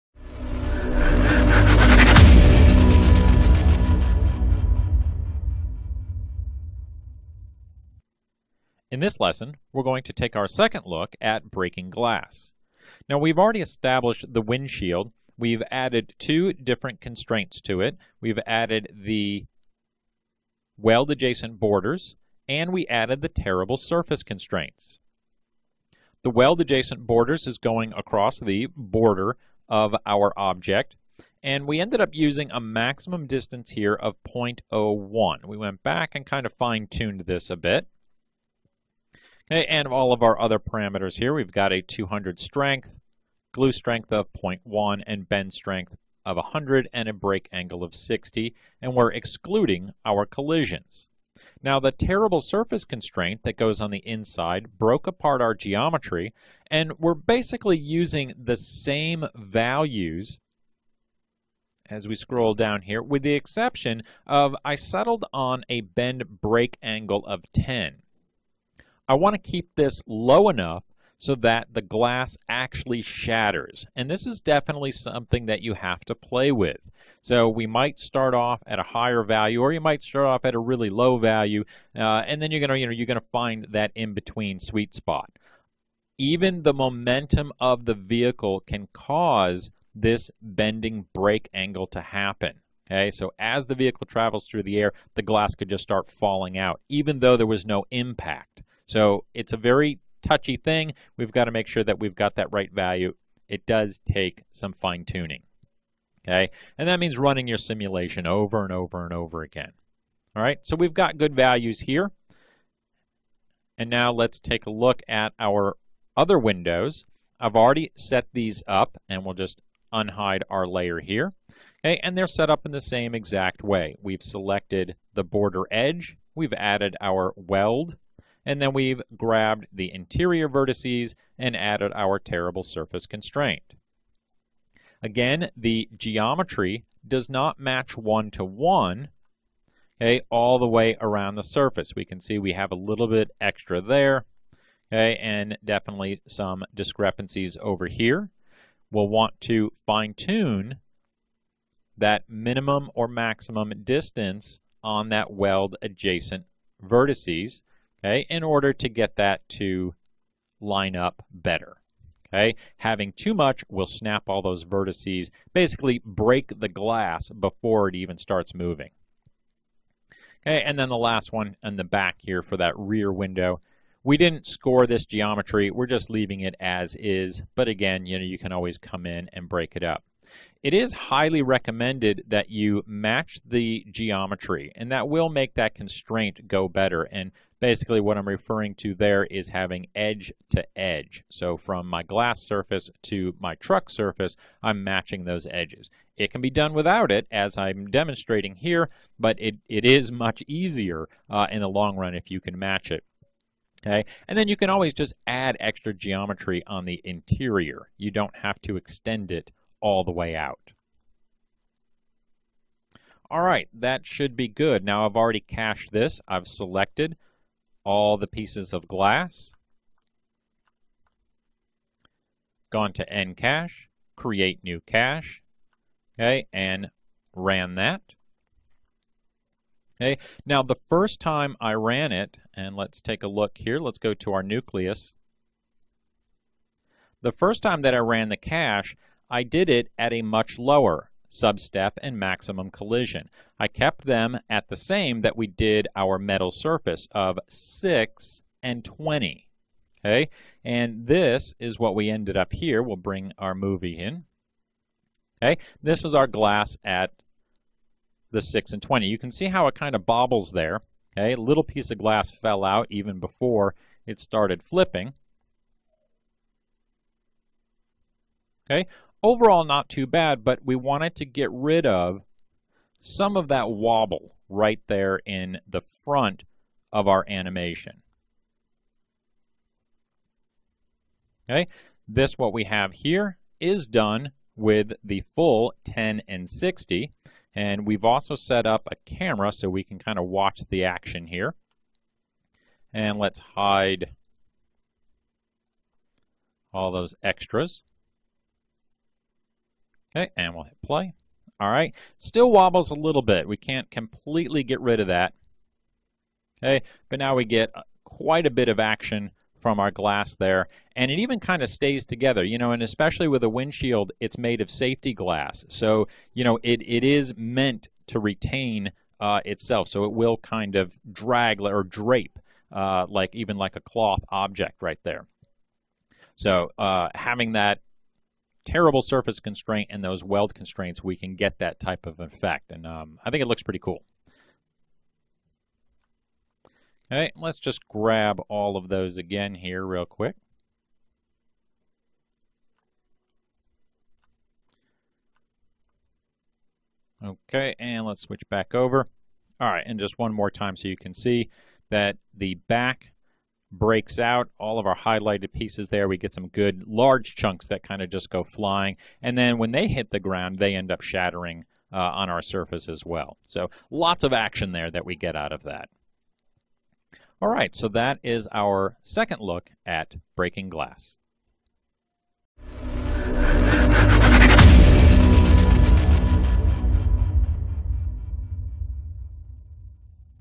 Autodesk Maya Tutorial | Car Crash Animation | Complete Course - Part 2
Topic# Simulating the Roll Cage
Description:
The focus of this Maya tutorial is to use nCloth to destroy parts of a vehicle. In this tutorial we'll take a vehicle and have it flip down a highway. We’ll simulate the effects in order to get an accurate and true-to-life representation of a real car crash. Then, we'll cover how to dent metal, shatter glass, and make parts break off.

By the end of the Maya training you'll have a solid understanding of how to use nCloth to destroy most objects.